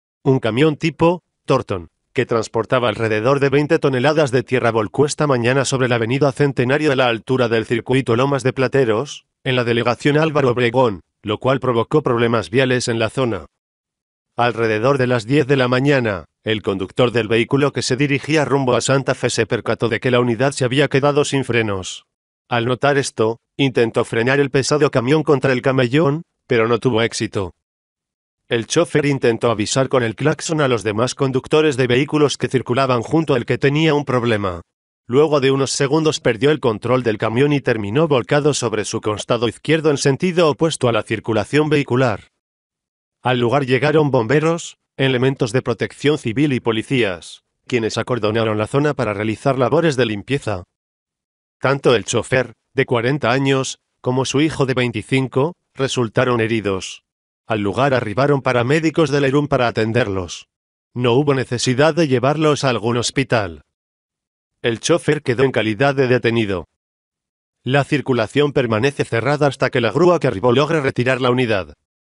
Vuelca camión sin frenos en la Álvaro Obregón CDMX
Un camión tipo ‘Torton’ que transportaba alrededor de 20 toneladas de tierra volcó esta mañana sobre la avenida Centenario a la altura del circuito Lomas de Plateros, en la delegación Álvaro Obregón, lo cual provocó problemas viales en la zona.

Alrededor de las diez de la mañana, el conductor del vehículo que se dirigía rumbo a Santa Fe se percató de que la unidad se había quedado sin frenos. Al notar esto, intentó frenar el pesado camión contra el camellón, pero no tuvo éxito.

El chofer intentó avisar con el claxon a los demás conductores de vehículos que circulaban junto a él que tenía un problema. Luego de unos segundos perdió el control del camión y terminó volcado sobre su costado izquierdo en sentido opuesto a la circulación vehicular.

Al lugar llegaron bomberos, elementos de protección civil y policías, quienes acordonaron la zona para realizar labores de limpieza.

Tanto el chofer, de 40 años, como su hijo de 25, resultaron heridos. Al lugar arribaron paramédicos del ERUM para atenderlos. No hubo necesidad de llevarlos a algún hospital.

El chofer quedó en calidad de detenido.

La circulación permanece cerrada hasta que la grúa que arribó logre retirar la unidad.